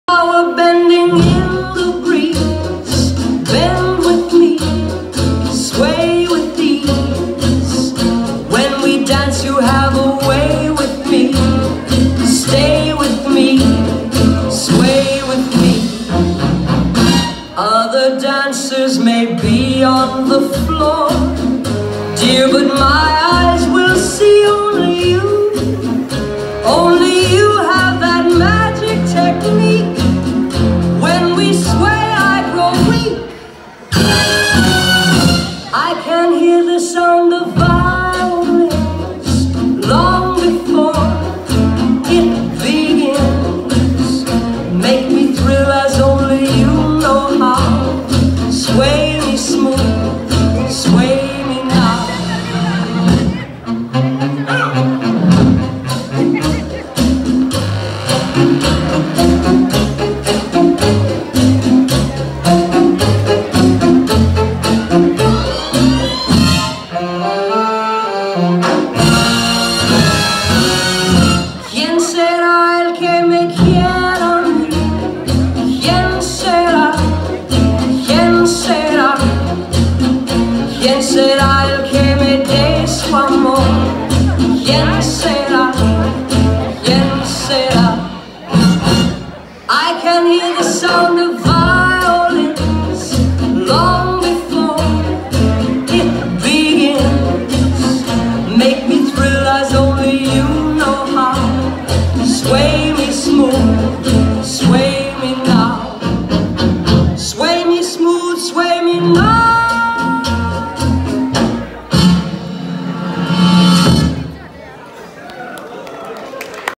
Feather belly dance Ivanna Dudka
New Year’s Eve feather  belly dance  performance Chicago 2018